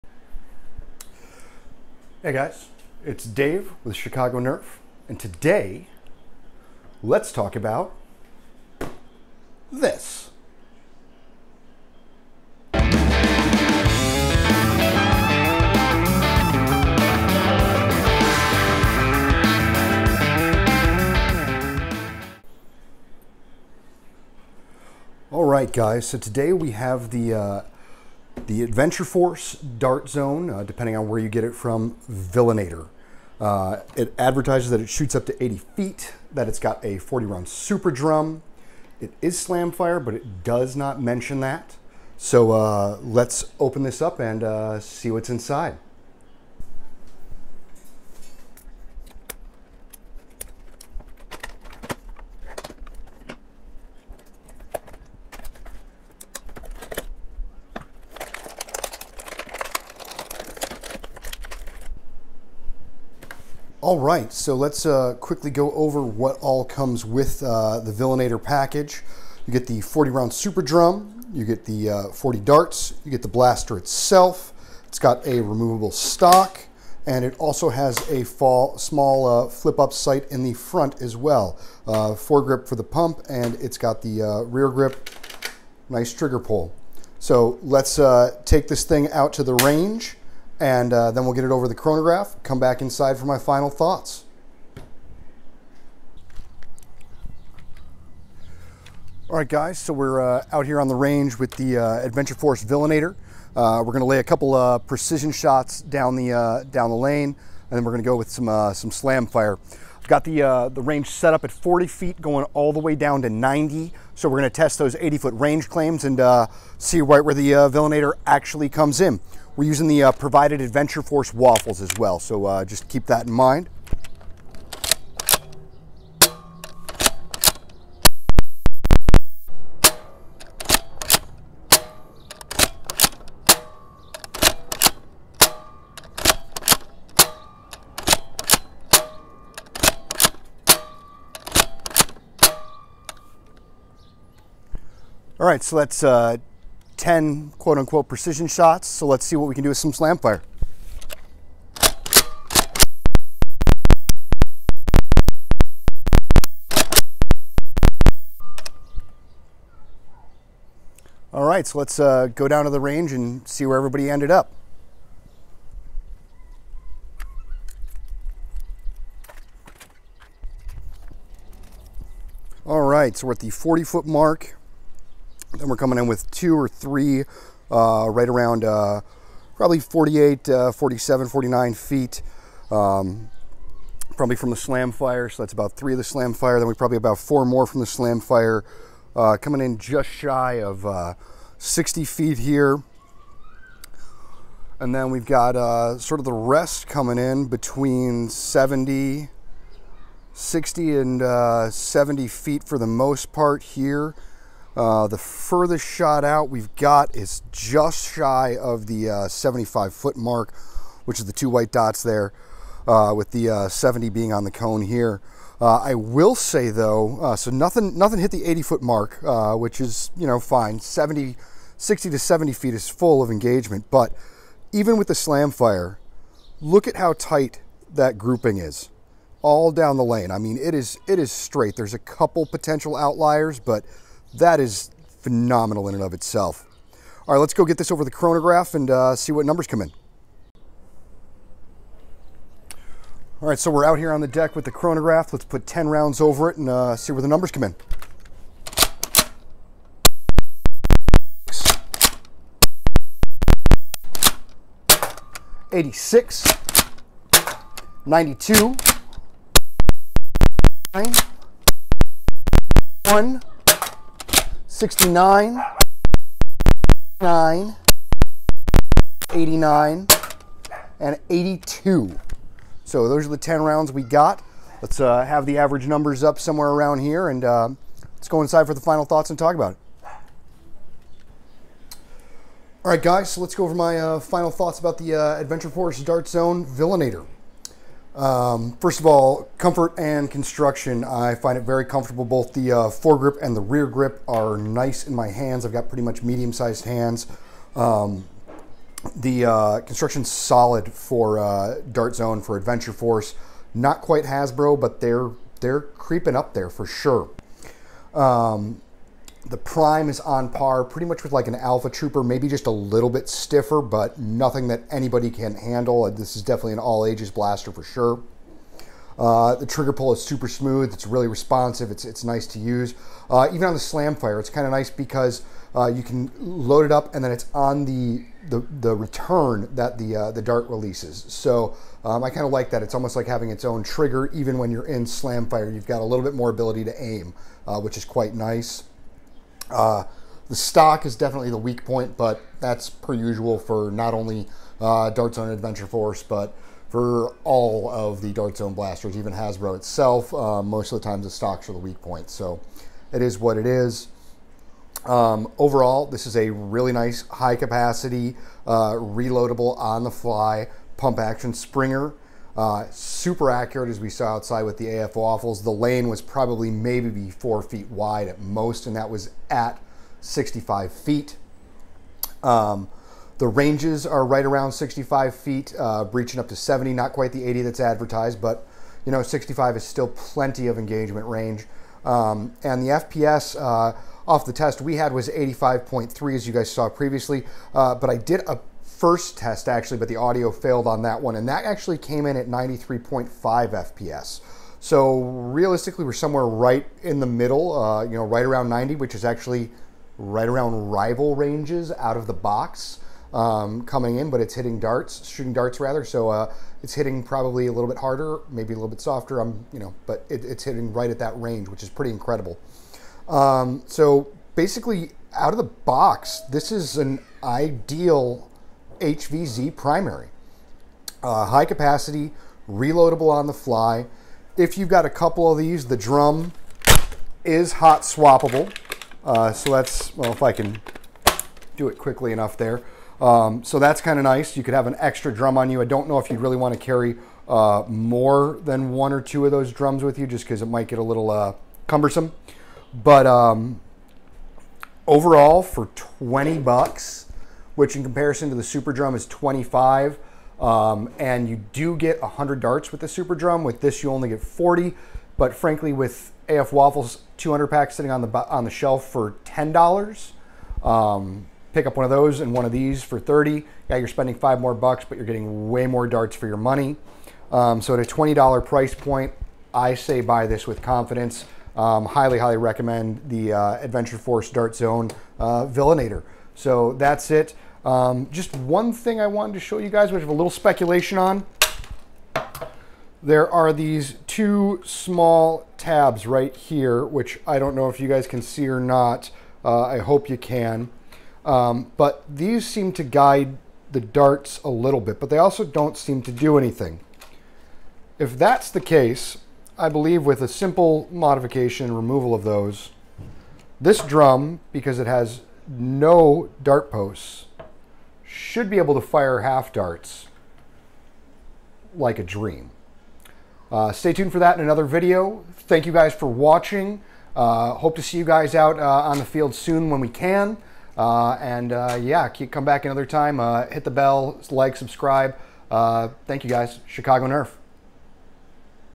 ADVENTURE FORCE VILLAINATOR IN DEPTH REVIEW & UNBOXING!!!
Adventure Force Villainator